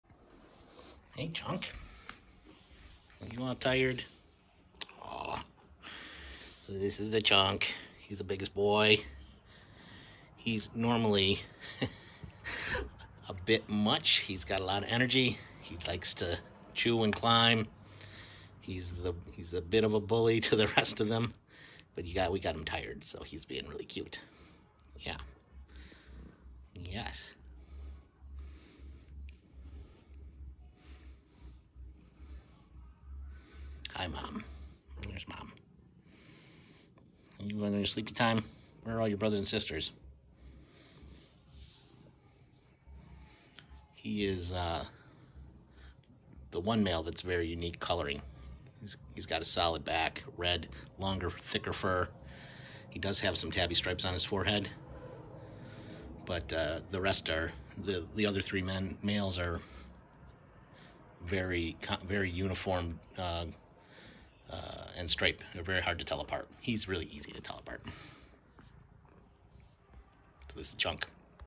Chonk
Foster kittens now available or adoption from the Hornell Humane Society. We adopted a pregnant Mom and now her babies are nearly weaned and ready to find permanent homes.